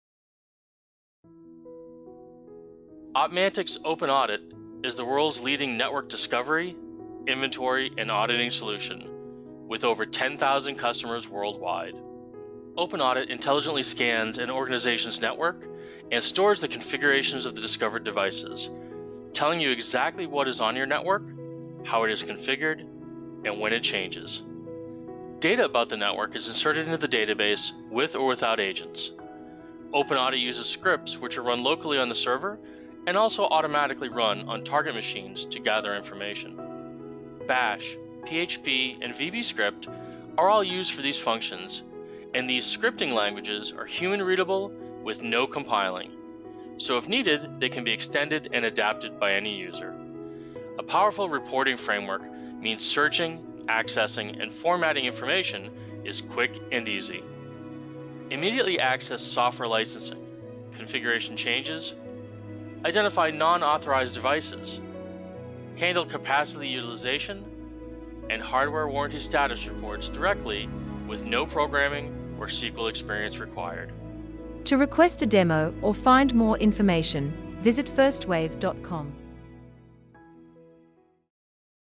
Learn about FirstWave's Open-AudIT
Open-AudIT is an application to tell you exactly what is on your network, how it is configured and when it changes. Most organizations cannot give accurate location data of their assets, Open-AudIT gives you this information in seconds.